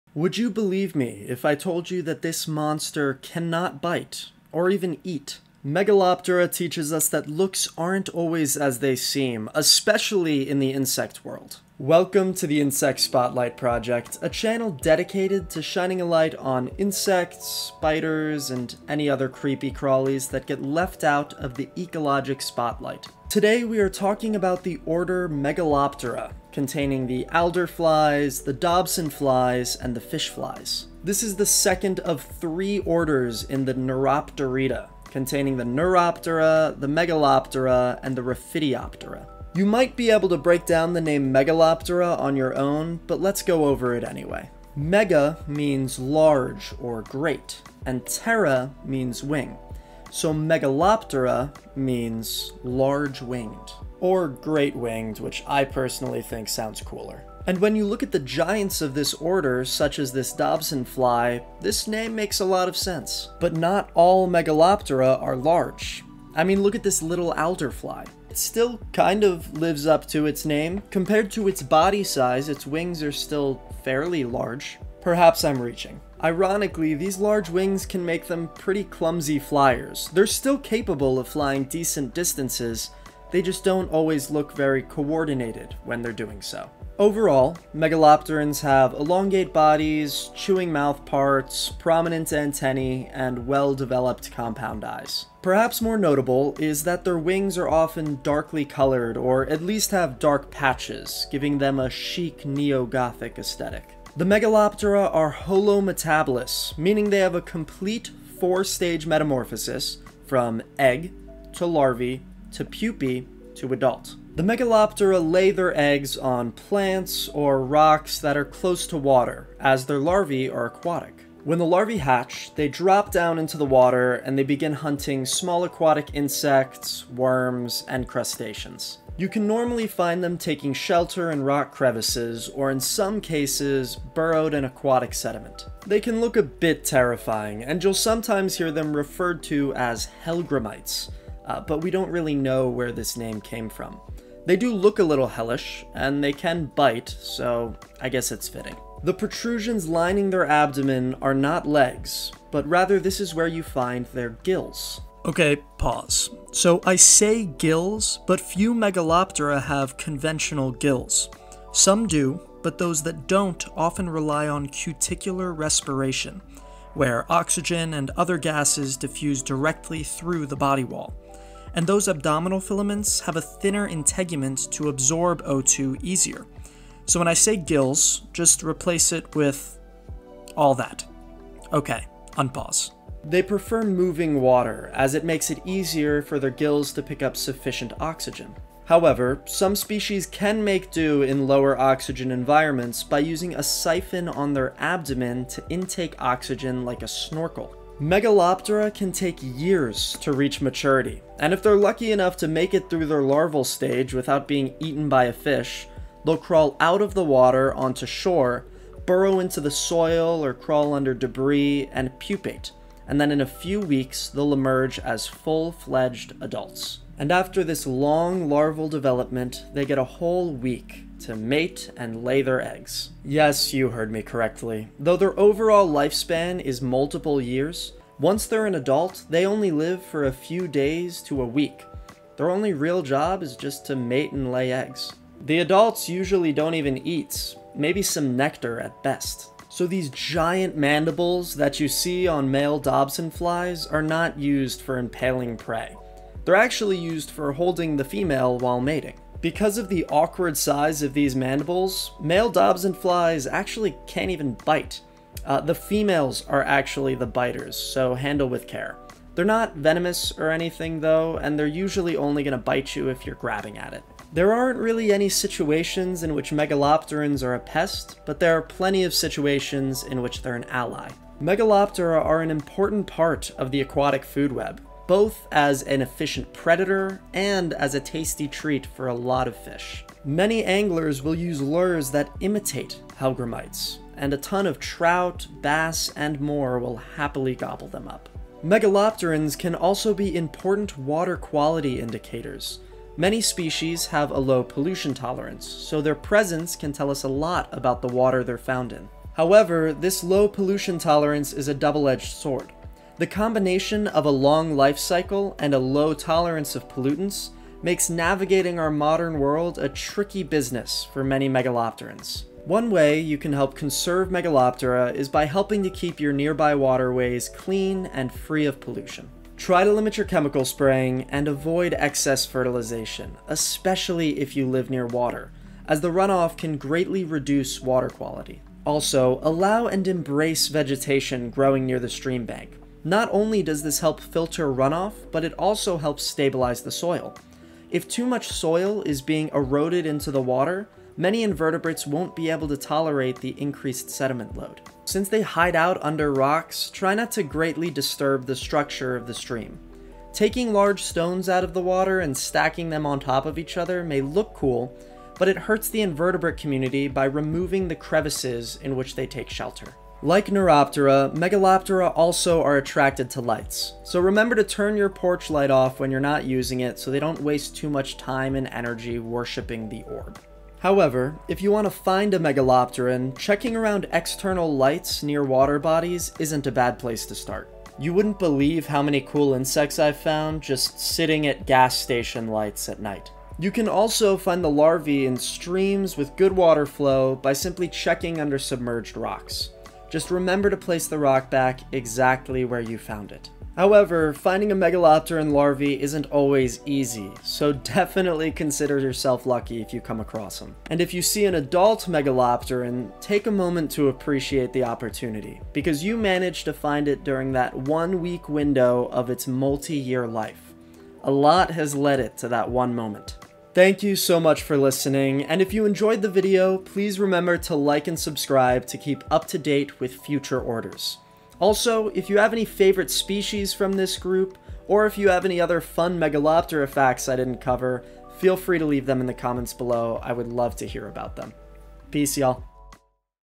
Megaloptera: Dobson, Alder, and Fishflies - Order Spotlight
🐟 Looks can be deceiving! Ruthless as larvae yet peaceful as adults, come learn about the puzzling life of Megaloptera. 🐟

🐟 Learn what they are, how to identify them, why they're so important, and why it can be so difficult to see one! 🐟